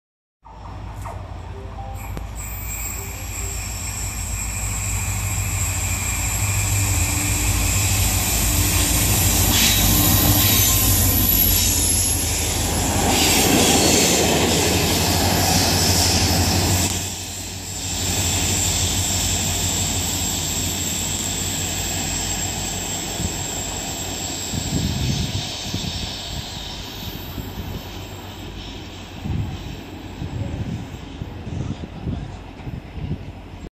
Renfe 333-379 con circulación especial
Éste tren se me ha echo muy raro que pasara porque siempre estuvo parado  en Tarragona